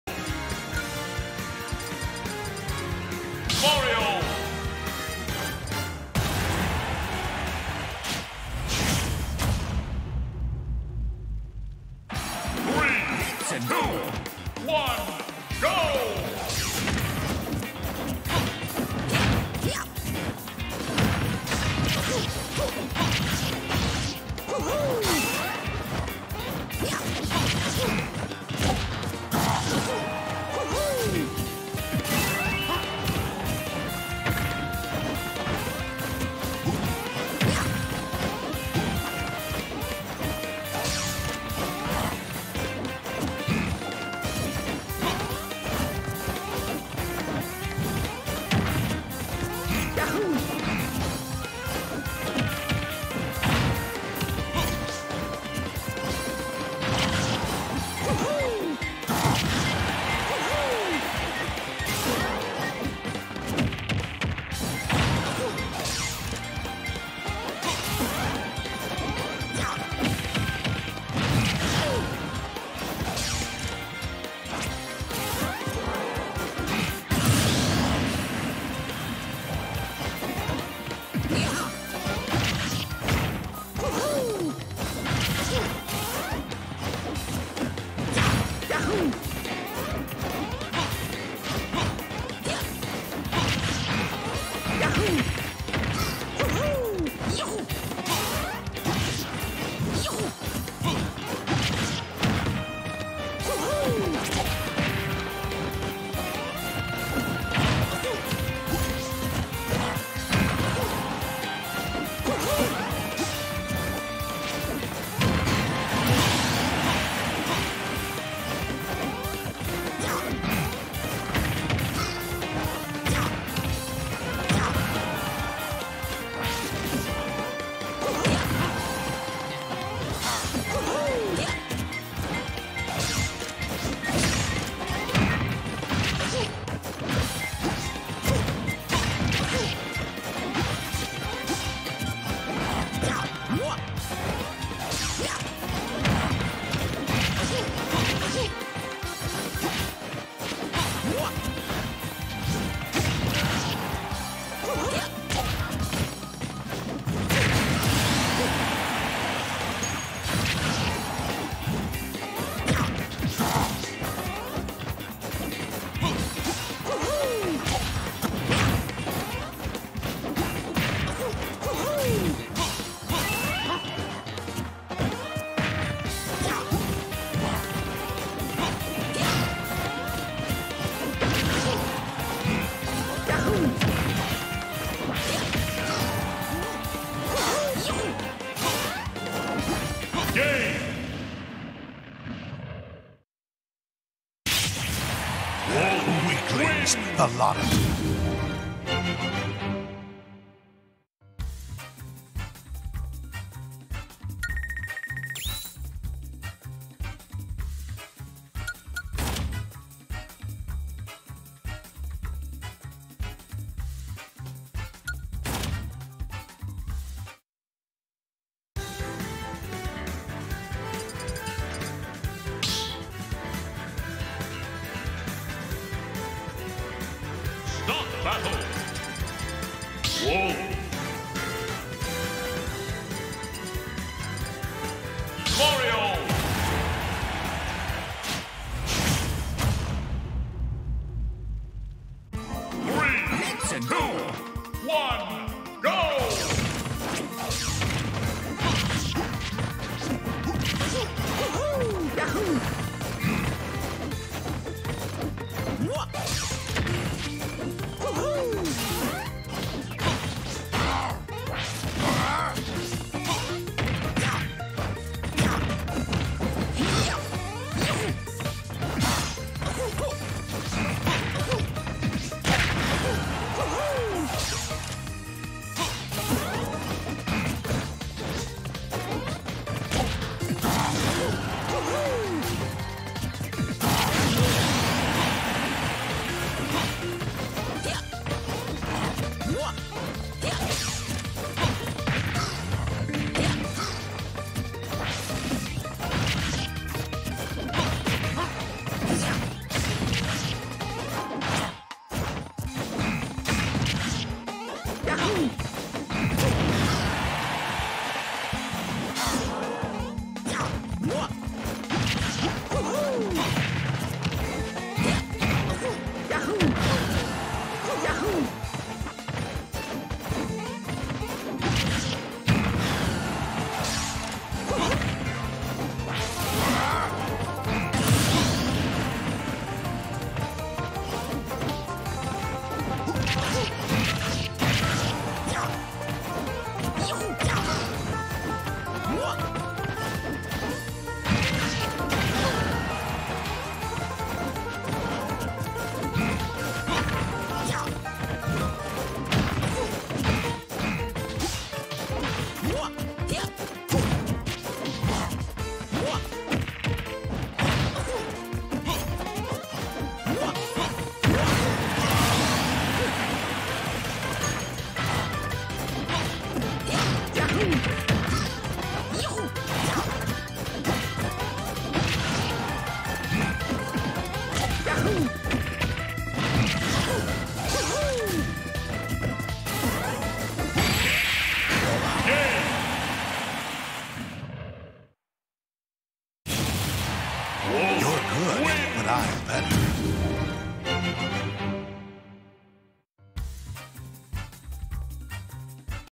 Finishing Touch Season 1 Week 4 | Winner's Quarters | [HHIG] ItsRod vs Bandz
Finishing Touch Season 1 Week 4
Tuesday, January 29th, 2018

Winner's Quarters
[HHIG] ItsRod (Wolf) vs Bandz (Mario)